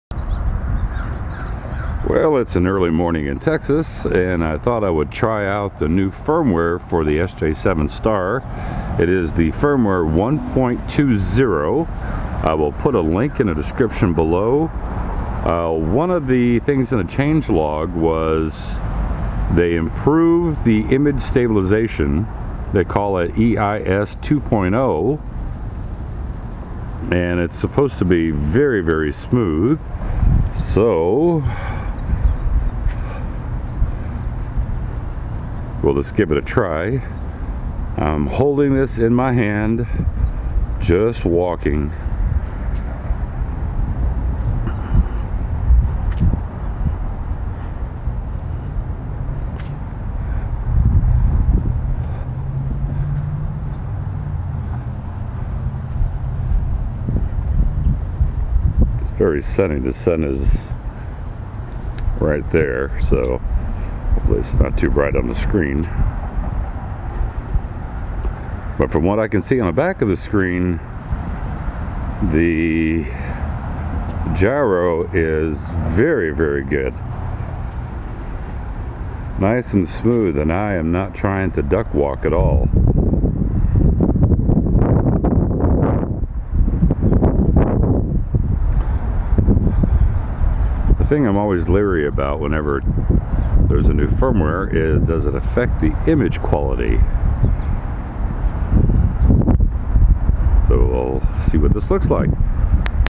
SJCAM SJ7 STAR firmware 1.20 gyro test 1080@60
SJCAM came out with a new firmware for the SJ7 STAR since they are coming out with their own handheld 3 axis gimbal, one of the things that are improved is the gyro/EIS 2.0 so I did a quick test of it this morning, looks good. Here is the change log for it.

SJ7 Star v1.20 Firmware Changelog

1. Adds Gimbal mode

2. EIS (Gyro Stabilization) updated to EIS2.0 (improved stabilization)

3. Adds distortion correction for the following resolutions:

(2.7K*30fps 4:3) (2.7K*30fps SV) (2.7K*30fps 16:9) (2.5k*30fps 16:9)

(1440P*60fps 4:3) (1440P*30fps 4:3) (1080P*60fps 16:9) (1080P*30fps 16:9) (1080P*60fps SV) (1080P*30fps SV) (960P*60fps 4:3) (960*30fps 4:3) (720P*60fps 16:9) (720P*30fps 16:9)

4. Adds 2160*24fps resolution

5. Optimizes Russian

6. Adds Loop Recording up to 3 minutes (default) (loop record this audio fix is coming).

7. Improves TV mode PAL 720P*120fps SV image quality.

8. Adds distortion correction (anti-fisheye)!

Gear I use